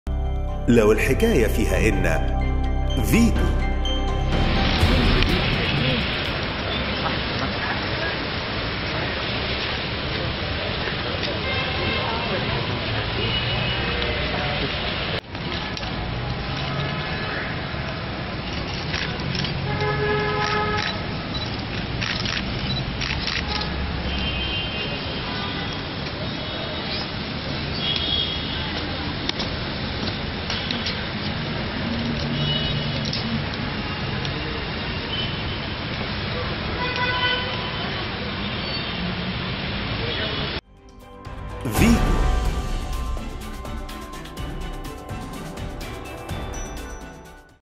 أبو شقة وشريف خالد في عزاء شقيق محمد شردى بعمر مكرم
فيتو - أبو شقة وشريف خالد في عزاء شقيق محمد شردى بعمر مكرم
ـــــــــــــــــــــــــــــ